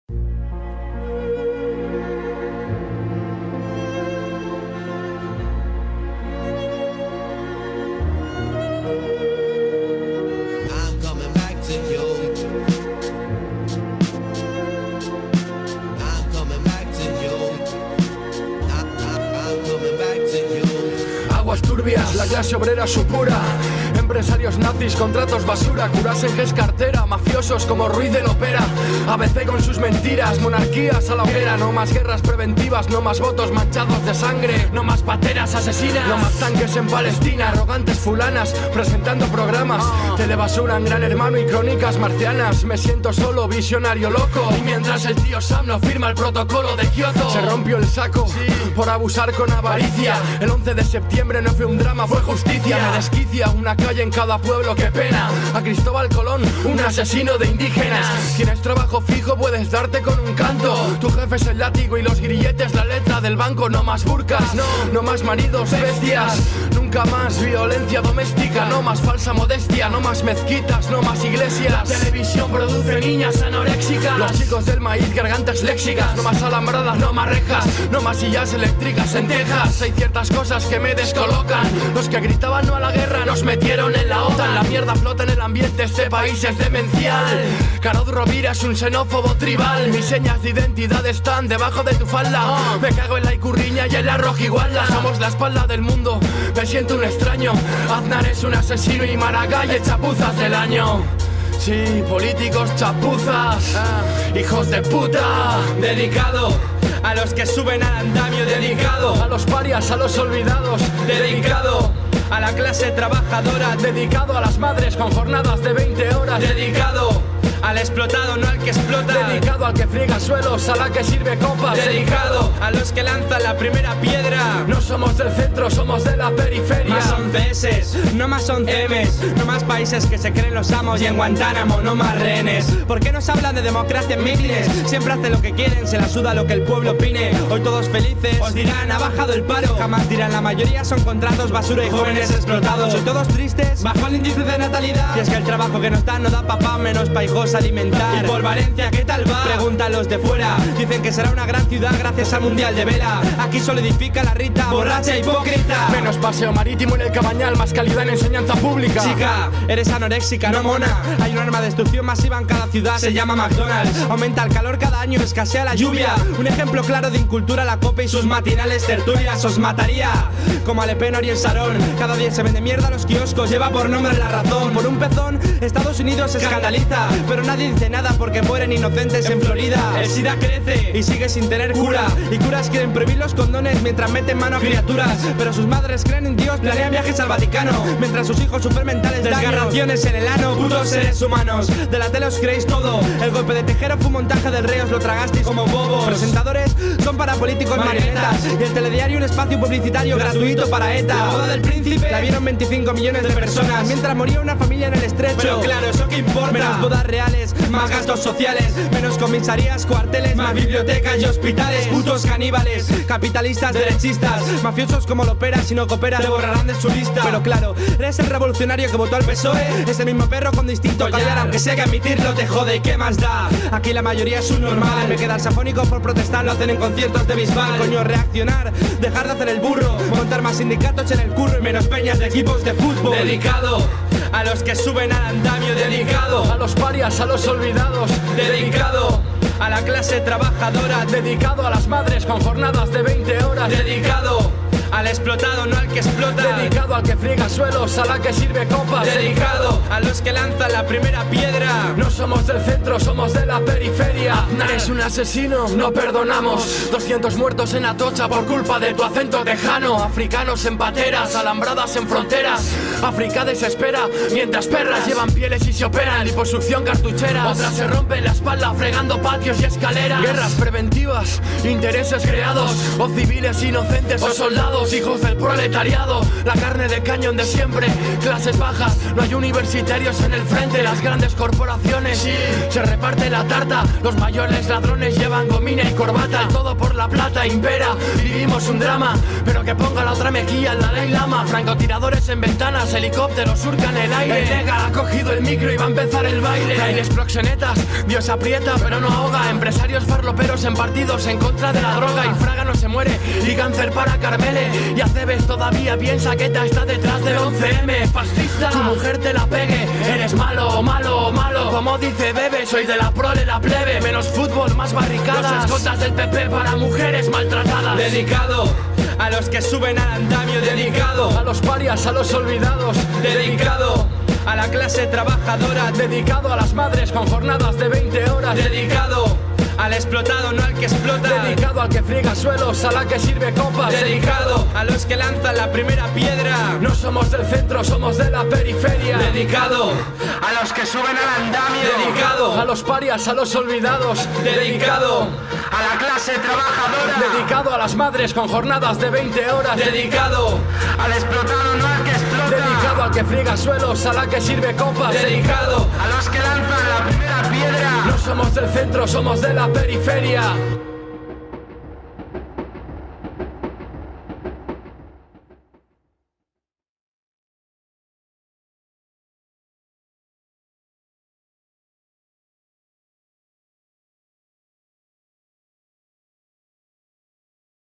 Video homenaje a los trabajadores ke luchan, a los ke tienen konciencia de klase, a los ke konservan aún las ansias de kambiarlo todo...kon un tema del grupo hip-hop valenciano "Los Chikos del Maíz". kon los mártires de chikago en la memoria; viva el 1° de mayo, vivan los trabajadores, viva la revolución!